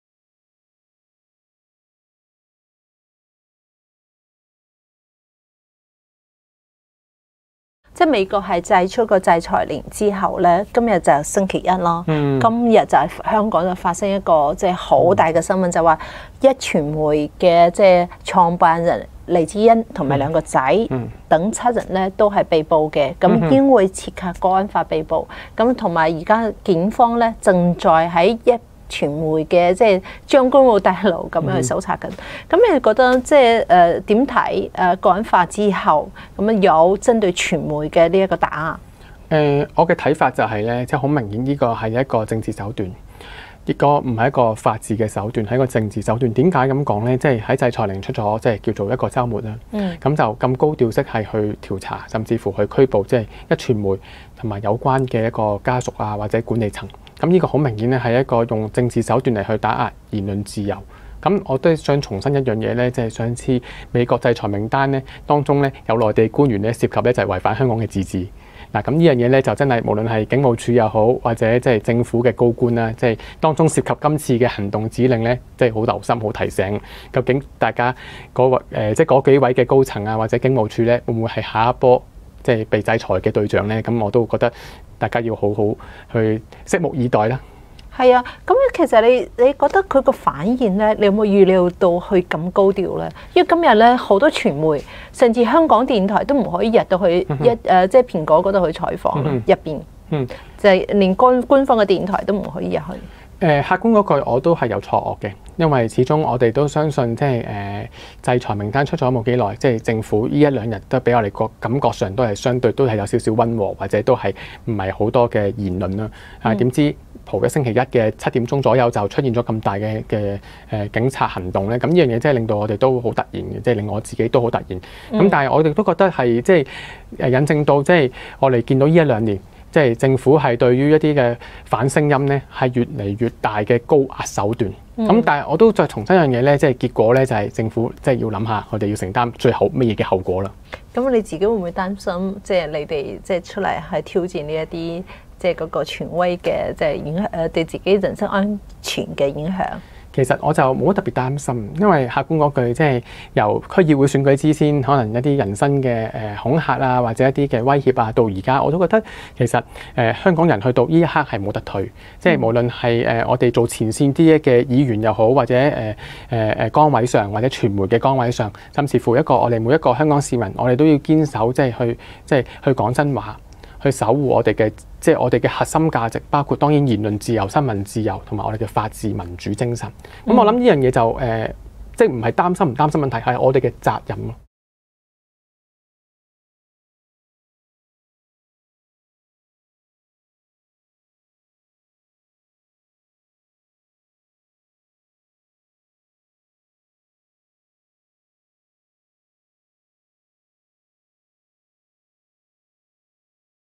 黎智英被捕事件令盧俊宇十分驚訝，他認為是用政治手段打壓言論自由，他表示無論他是否是個區議員，首先是香港人，堅守立場跟說真話是一定要做的事
中共強推國安法的結局如何🤔？香港成了中美二龍奪珠的主戰場🥊，勝負將如何⚔️？！